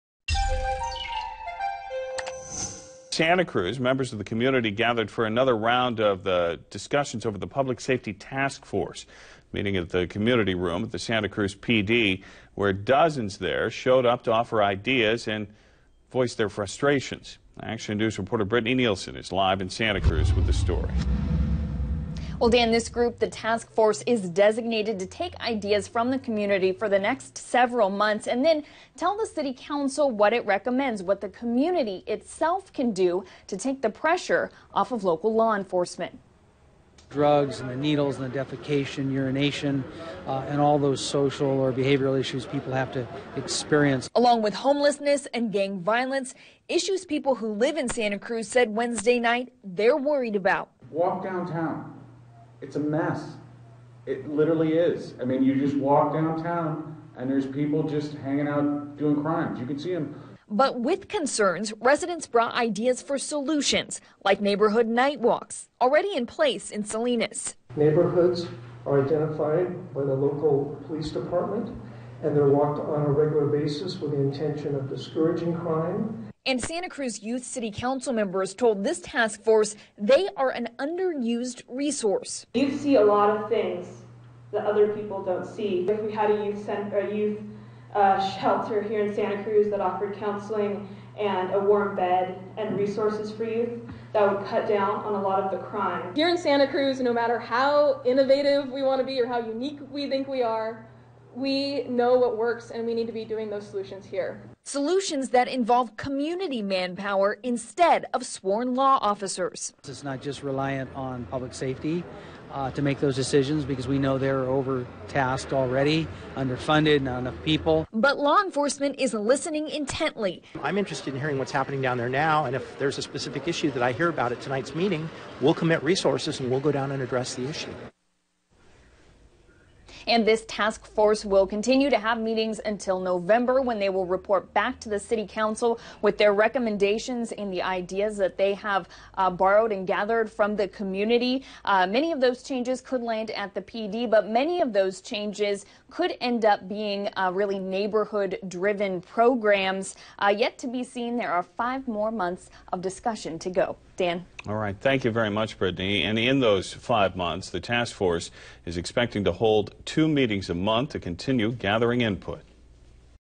Santa Cruz Public Safety Meeting
Community members gathered for another round of the public safety task force. They met at the community room at the Santa Cruz police department where dozens of community members showed up to offer ideas, and frustrations.